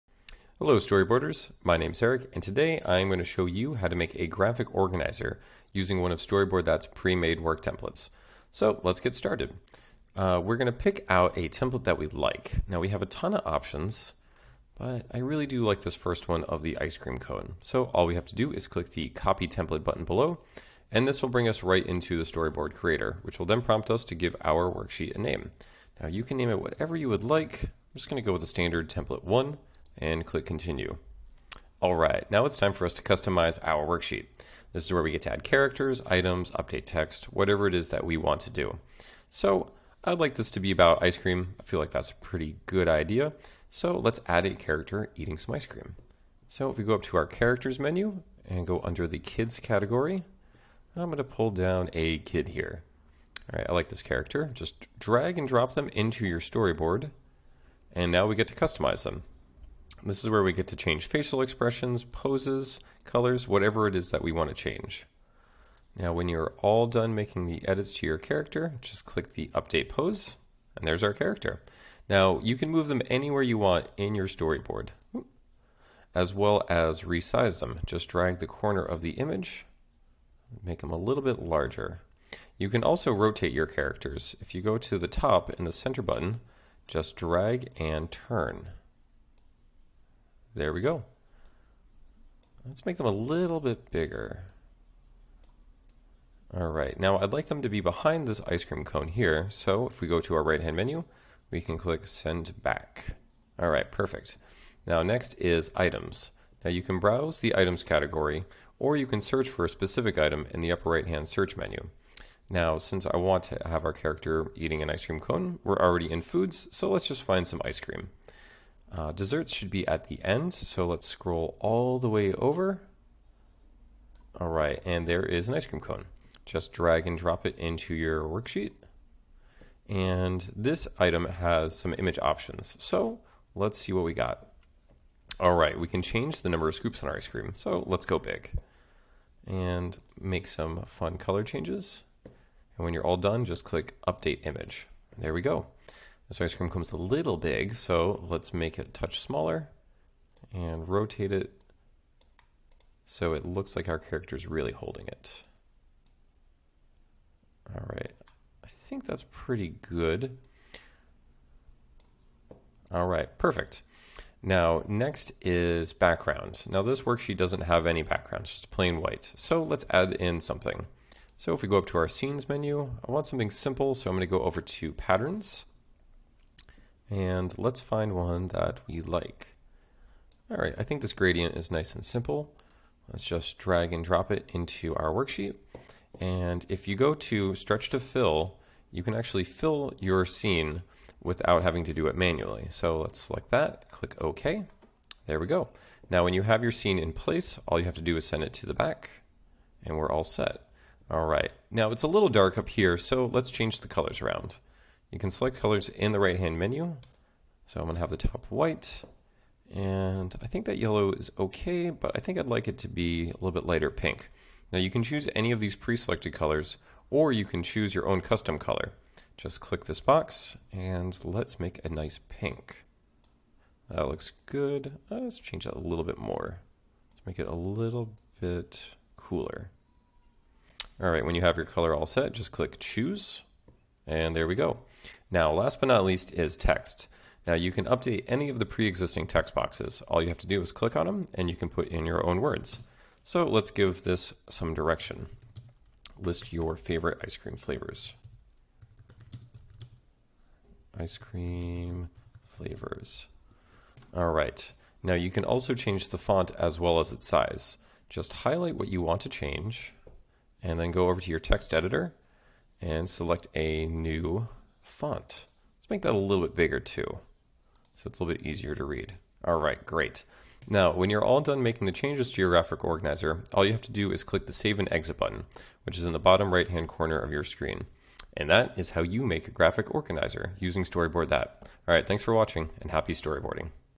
How to make a Graphic Organizer Worksheet in the Storyboard Creator
Eric will teach you how to make a Graphic Organizer Worksheet in the Storyboard Creator.